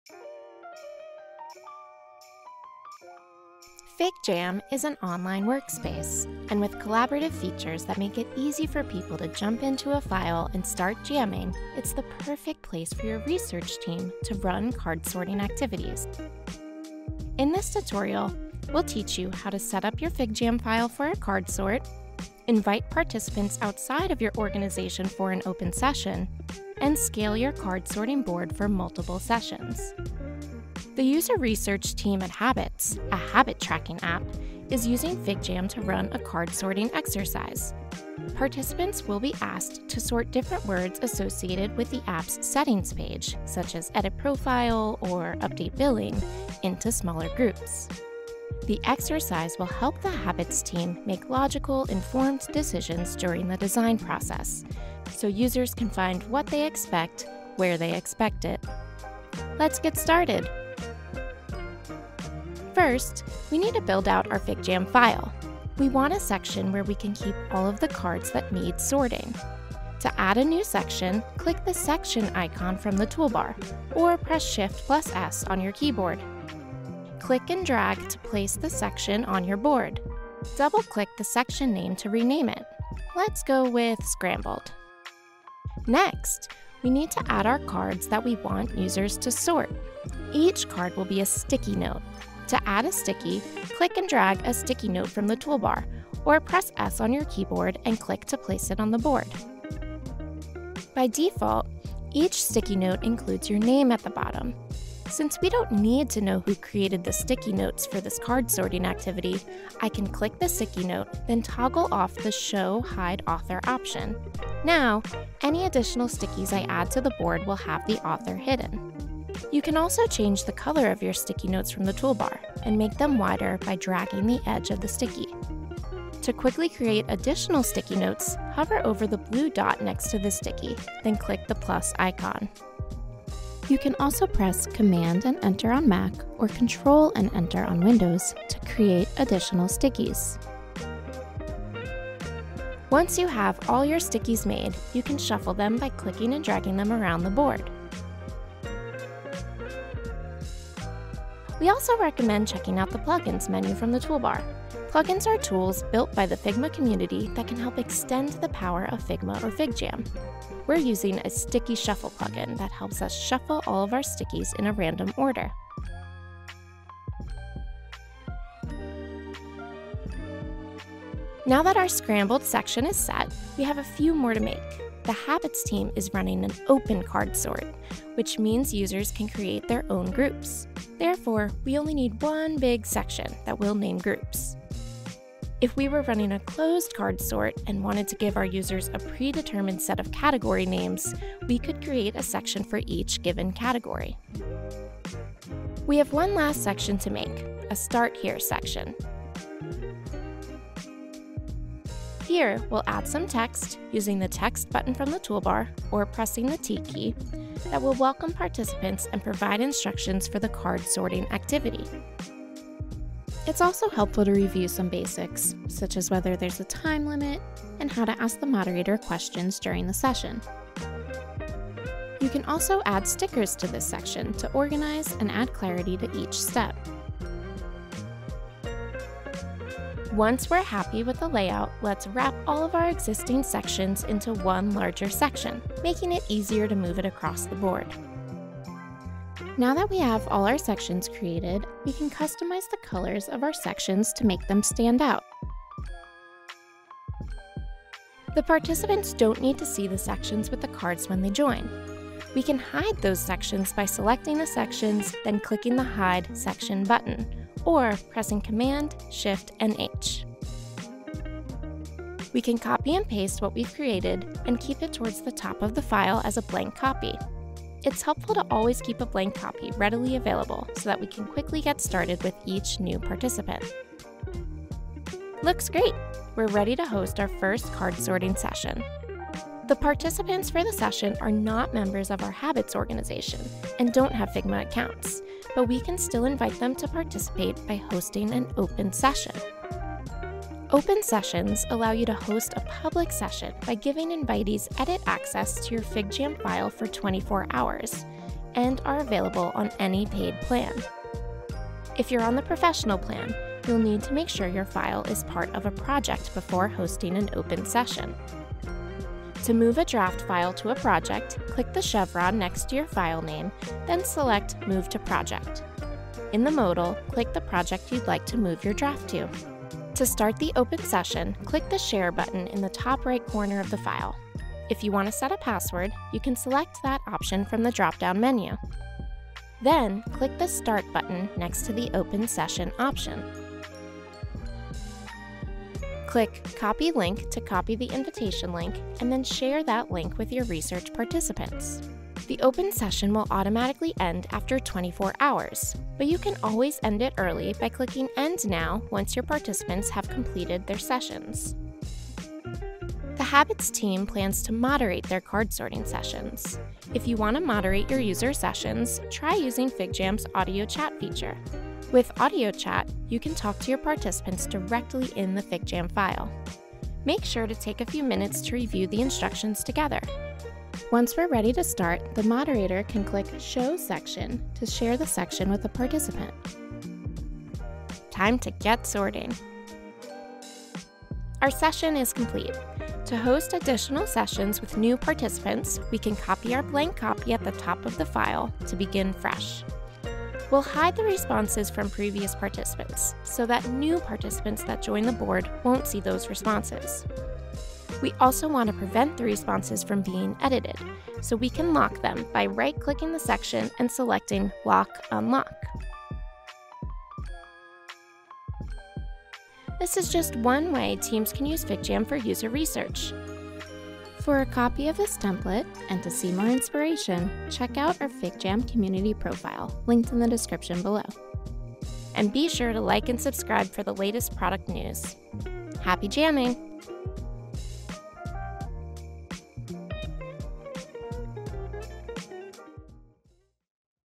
FigJam for User Research Card Sorting
FigJam is the perfect card sorting tool for research teams. In this tutorial, we’ll teach you how to create a card sorting template and how to use FigJam to run a card sorting activity with your participants.

00:00 Intro
00:31 What is a card sort activity?
01:00 Create a section
01:25 Add stickies
02:32 Plugins
02:48 Prepare your template
03:16 Write instructions for participants
03:40 Stickers
03:58 Customize section colors
04:06 Hide sections
04:21 Copy your template for reuse
04:39 Invite participants
05:54 Moderate a session
06:48 Outro